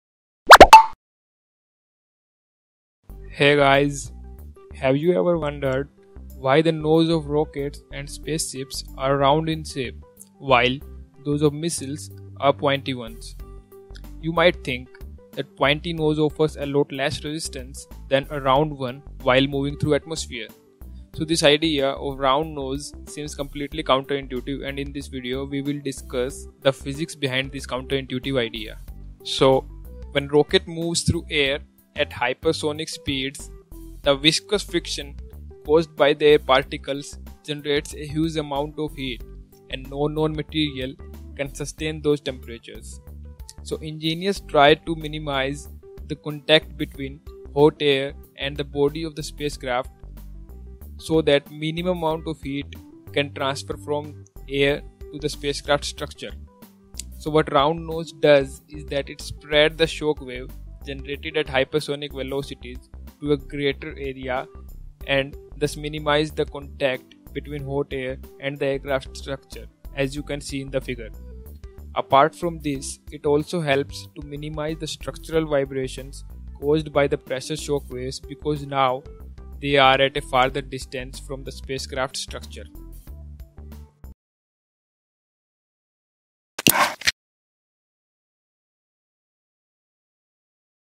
WHY Rockets Have Round Nose While Missiles Have Sharp Nose-Explained
We all know that a sharp object move more easily through air as compared to a bluff body. So have you ever though why Spaceships have round nose while missiles  have sharp nose even when the rocket moves much faster than missile. Find out in this video.

Thumbnail Credit: Scout.com